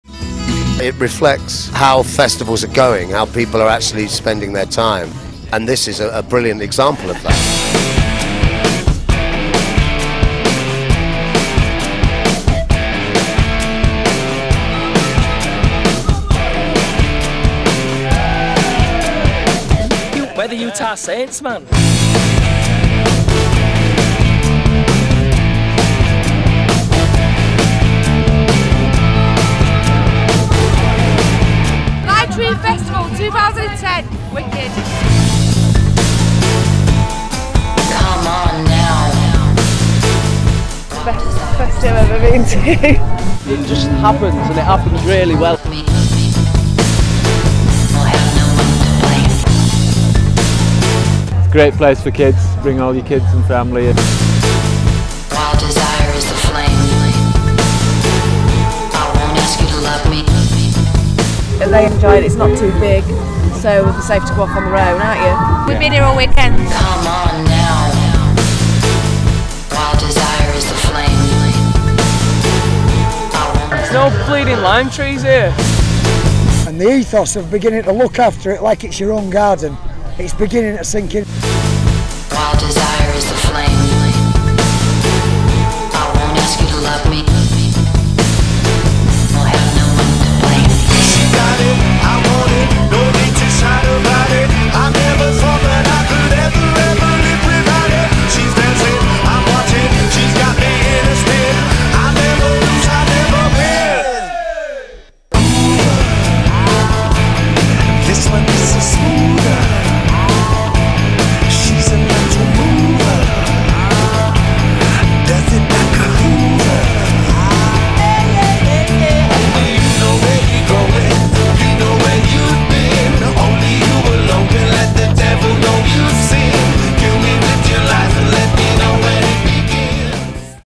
Limetree Festival
A short promo  to draw attention to the fabulous Limetree Festival nr Ripon.